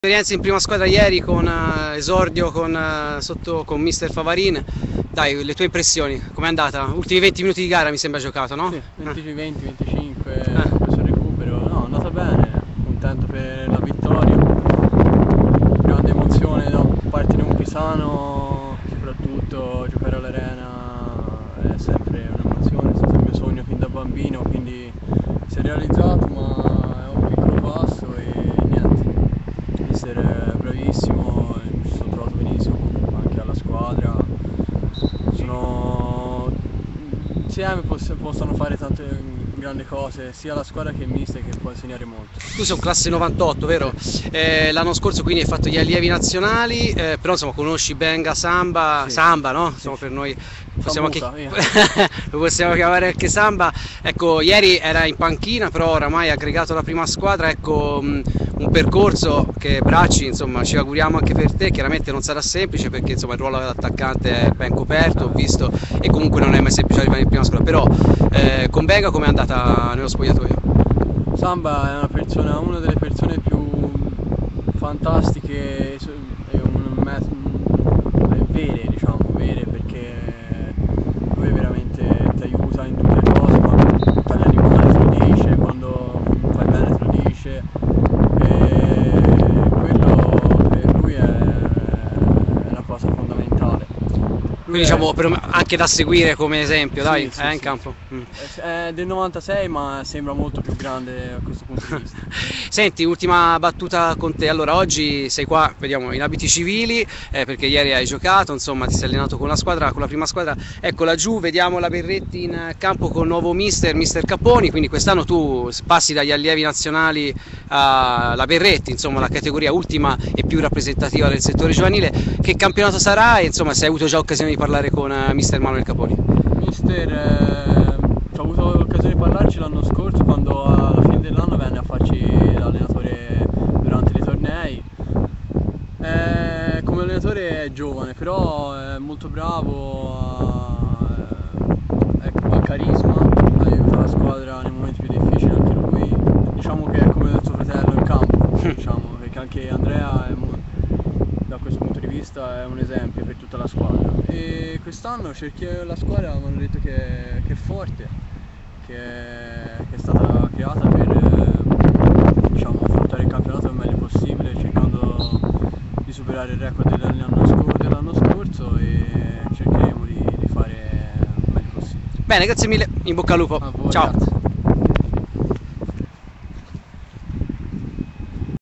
Matteo Bracci ('98)
Intervista a Mattepo Bracci il giorno dopo l'esordio in prima squadra alle dipendenze di mister Favarin nel match di Coppa Italia Tim contro il Sestri Levante - Captured Live on Ustream with the Ustream Mobile App